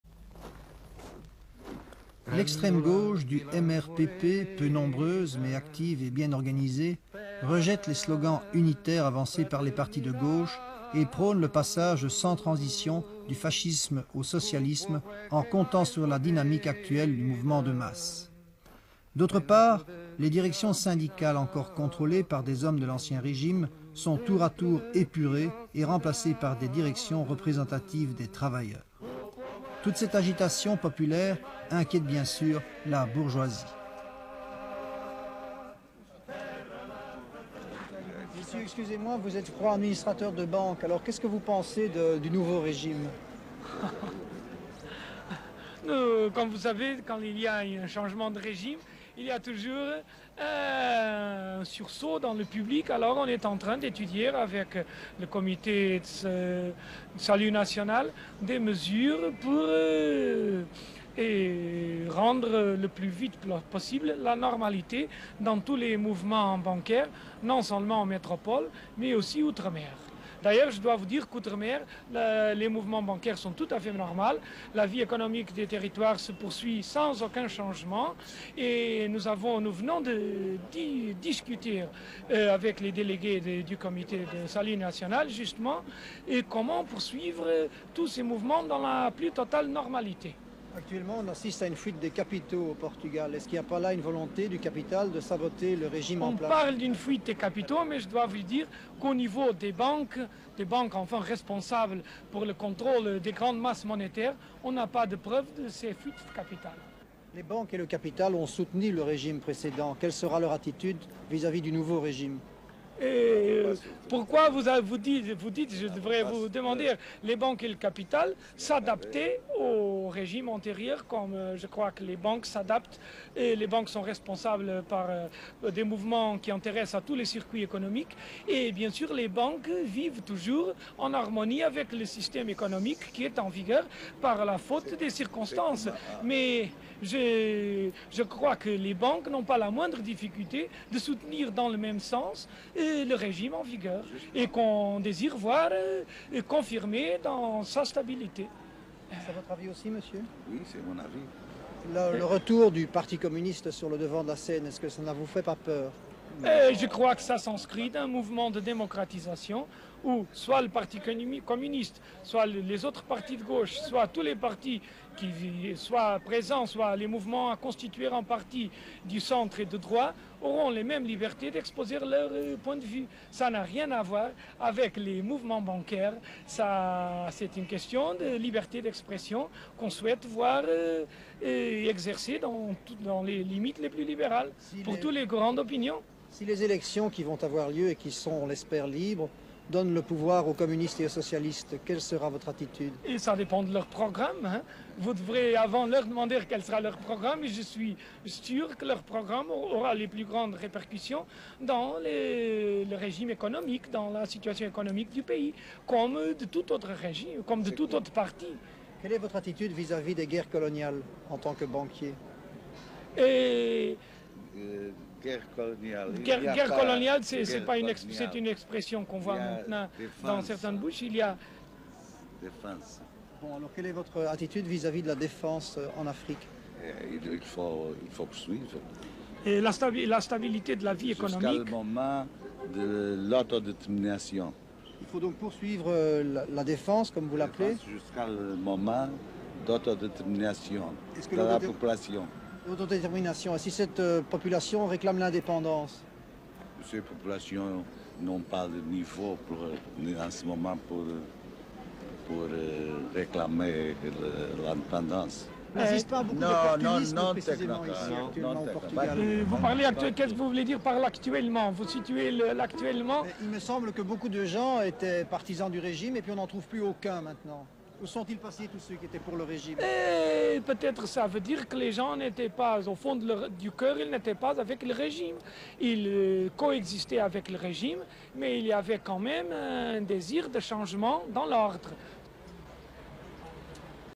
La révolution et les banquiers
Interview de banquiers au lendemain de la révolution des œillets du 25 avril 1974 au Portugal. Le document dont cette vidéo est extraite a été supprimé de Yt et je n'ai plus son titre. On peut remarquer la rapidité avec laquelle ces banquiers cherchent à récupérer la situation, à grand renfort de langue de bois. "Les banques n'ont pas la moindre difficulté de soutenir [autant que le précédent régime] le nouveau régime en vigueur qu'on désir voir confirmé dans sa stabilité."
Il ne faudrait pas parler de guerre coloniale mais de "défense" (langage orwellien !) "La population [des colonies africaines] n'a pas le niveau en ce moment pour réclamer l'indépendance" est-il affirmé.

Ne laissons pas les hommes de pouvoir écrire la constitution, écrivons dans notre constitution citoyenne les contrôles rendant impossible toute récupération.